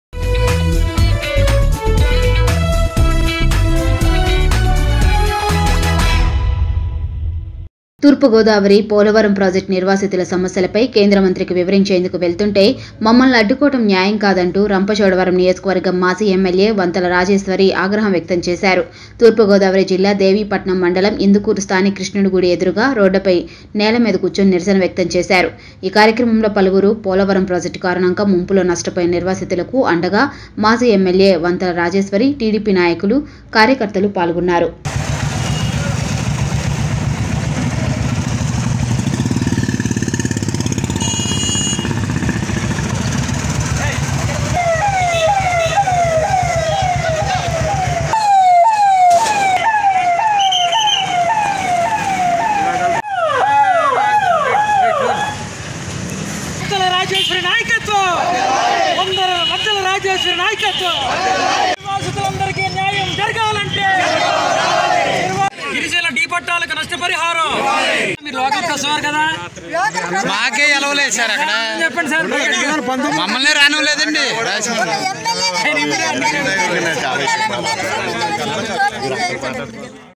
సమస్యలపై కేంద్ర మంత్రికి వివరించేందుకు వెళుతుంటే అడ్డుకోవడం సరి కాదు:Ex MLA వంతల రాజేశ్వరిTRUTHNEWS
@truthnewstelugu సమస్యలపై కేంద్ర మంత్రికి వివరించేందుకు వెళుతుంటే అడ్డుకోవడం సరి కాదు:Ex MLA వంతల రాజేశ్వరిTRUTHNEWS